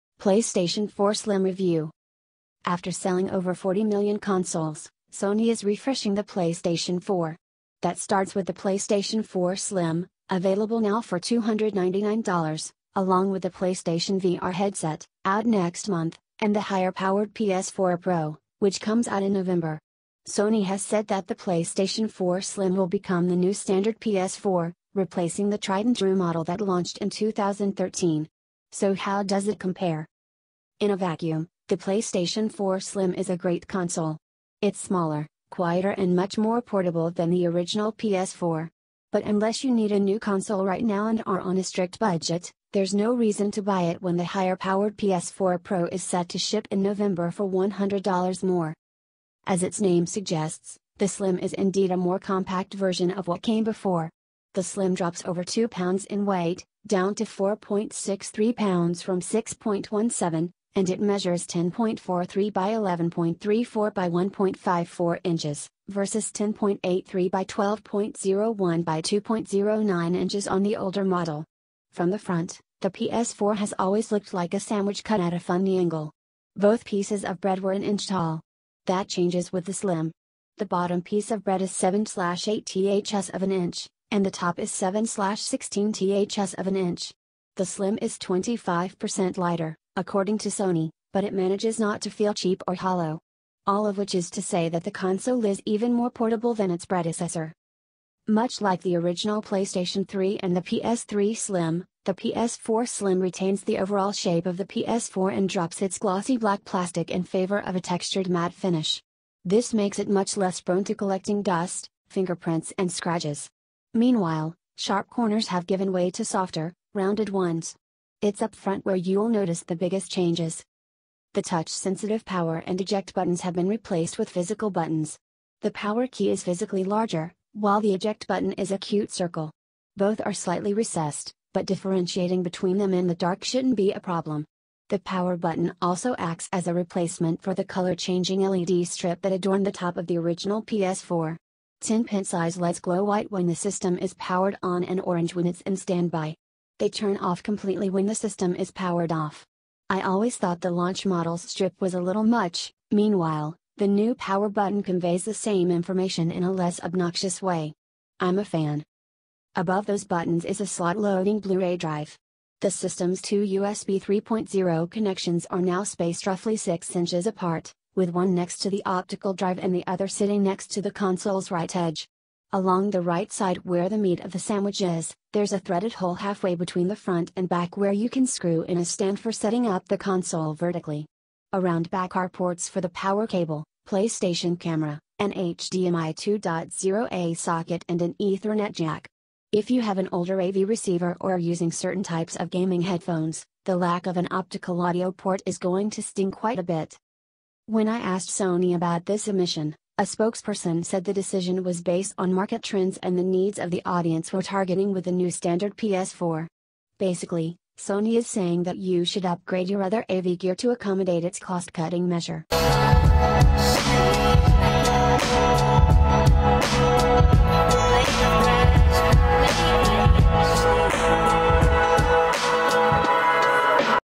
PlayStation 4 Slim review
After selling over 40 million consoles, Sony is refreshing the PlayStation 4. That starts with the PlayStation 4 Slim, available now for $299, along with the PlayStation VR headset (out next month) and the higher-powered PS4 Pro, which comes out in November. Sony has said that the PlayStation 4 Slim will become the new standard PS4, replacing the tried-and-true model that launched in 2013. So how does it compare?

In a vacuum, the PlayStation 4 Slim is a great console. It's smaller, quieter and much more portable than the original PS4. But unless you need a new console right now and are on a strict budget, there's no reason to buy it when the higher-powered PS4 Pro is set to ship in November for $100 more.

As its name suggests, the Slim is indeed a more compact version of what came before. The Slim drops over 2 pounds in weight, down to 4.63 pounds from 6.17, and it measures 10.43 x 11.34 x 1.54 inches, versus 10.83 x 12.01 x 2.09 inches on the older model. From the front, the PS4 has always looked like a sandwich cut at a funny angle. Both pieces of "bread" were an inch tall. That changes with the Slim. The bottom piece of "bread" is 7/8ths of an inch, and the top is 7/16ths of an inch. The Slim is 25 percent lighter, according to Sony, but it manages not to feel cheap or hollow. All of which is to say that the console is even more portable than its predecessor.
Much like the original PlayStation 3 and the PS3 Slim, the PS4 Slim retains the overall shape of the PS4 and drops its glossy black plastic in favor of a textured matte finish. This makes it much less prone to collecting dust, fingerprints and scratches. Meanwhile, sharp corners have given way to softer, rounded ones. It's up front where you'll notice the biggest changes.

The touch-sensitive power and eject buttons have been replaced with physical buttons. The power key is physically larger, while the eject button is a cute circle. Both are slightly recessed, but differentiating between them in the dark shouldn't be a problem. The power button also acts as a replacement for the color-changing LED strip that adorned the top of the original PS4. Ten pin-size LEDs glow white when the system is powered on and orange when it's in standby. They turn off completely when the system is powered off. I always thought the launch model's strip was a little much; meanwhile, the new power button conveys the same information in a less obnoxious way. I'm a fan.

Above those buttons is a slot-loading Blu-ray drive. The system's two USB 3.0 connections are now spaced roughly 6 inches apart, with one next to the optical drive and the other sitting next to the console's right edge. Along the right side where the "meat" of the sandwich is, there's a threaded hole halfway between the front and back where you can screw in a stand for setting up the console vertically. Around back are ports for the power cable, PlayStation Camera, an HDMI 2.0a socket and an Ethernet jack. If you have an older A/V receiver or are using certain types of gaming headphones, the lack of an optical audio port is going to sting quite a bit.

When I asked Sony about this omission, a spokesperson said the decision was based on "market trends and the needs of the audience we're targeting with the new standard PS4." Basically, Sony is saying that you should upgrade your other A/V gear to accommodate its cost-cutting measure.